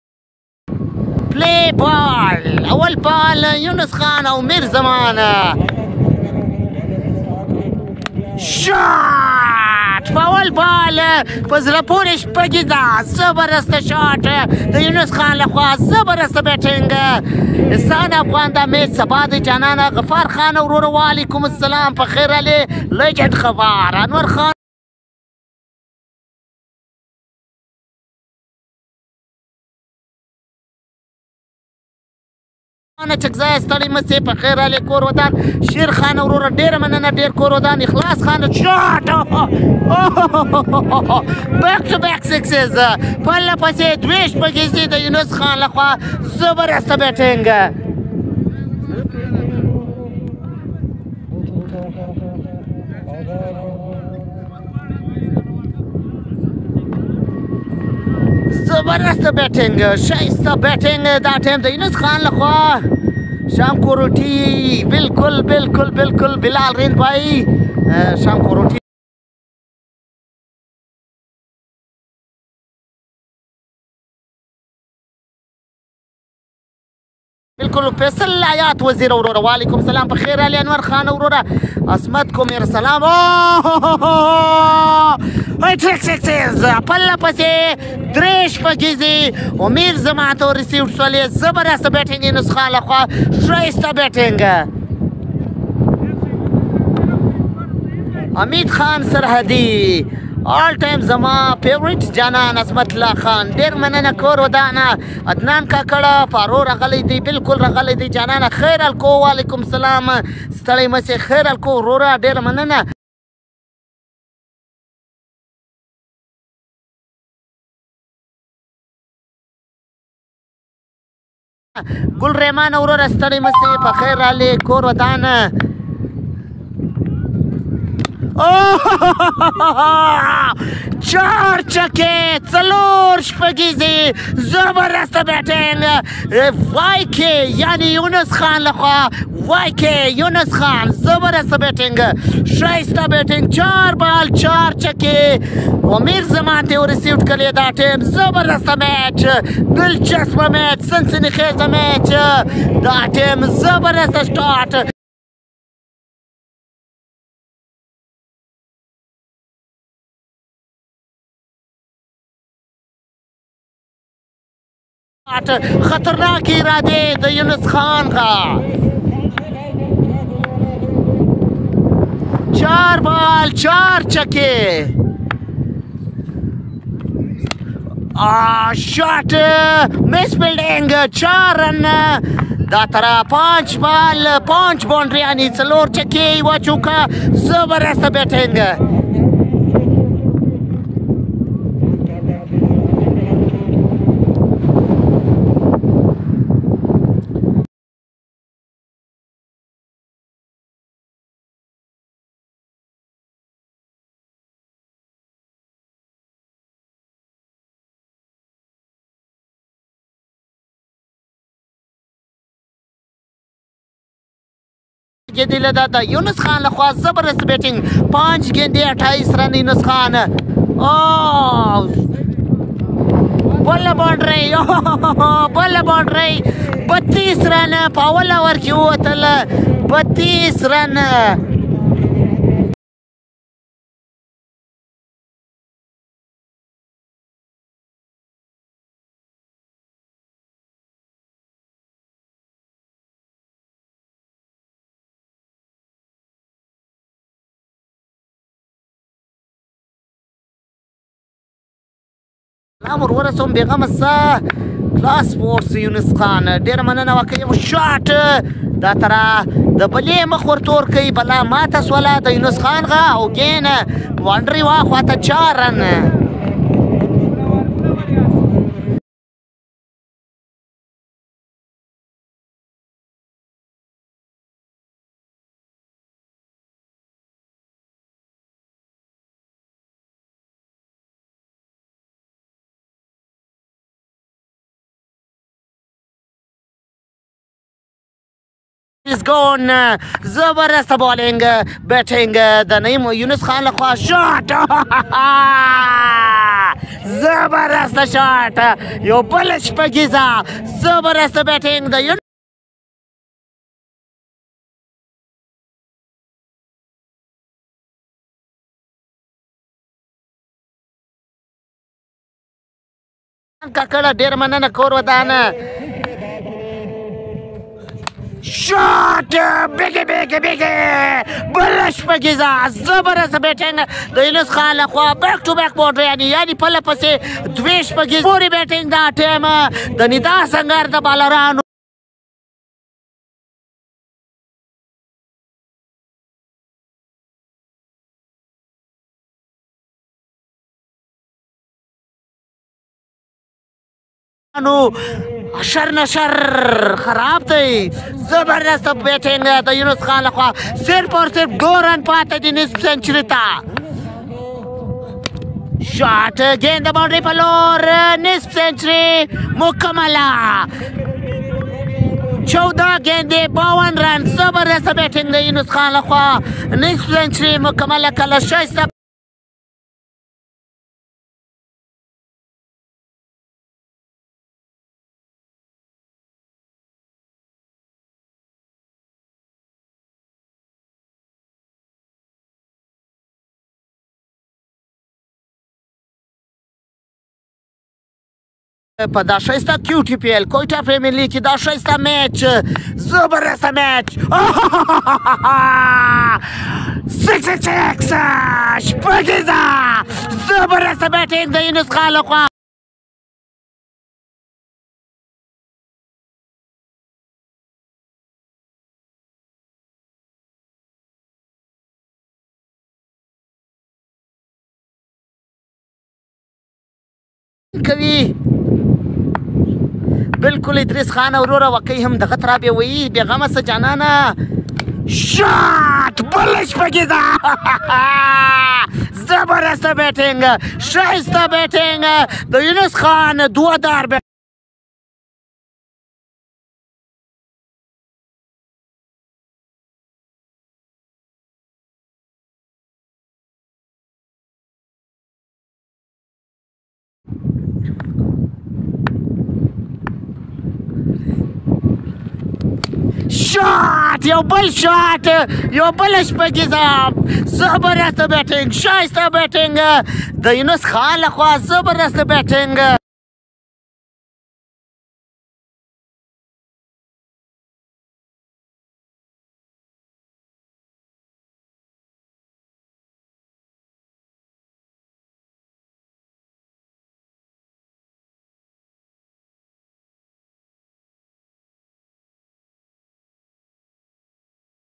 Younas Khan Great Inning 70 in Just 20 Balls in QPL | Quetta TapeBall Youngster Younas Khan Batting
Quetta Tape Ball Young Talented Barsman Younas Khan Great Batting in QPL